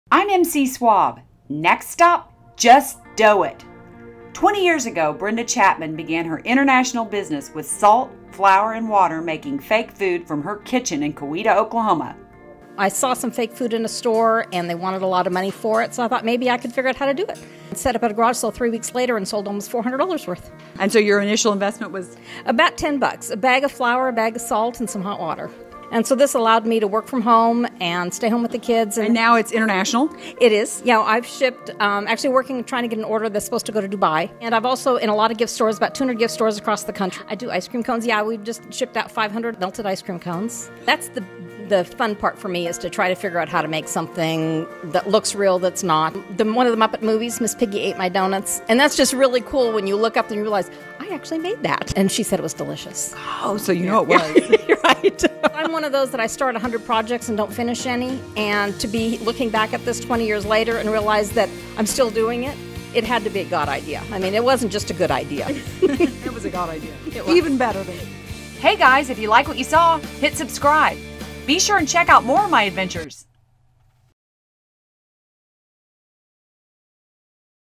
Fake Food by Fly the Coop MC Swab
Brenda Chapman is a momprenuer who began her fake food business, Just Dough it!, with flour, salt and water.  Now she sells these tasty look alike all over the world!

@flythecoop on facebook, twitter and instagram
@mcswab on snapchat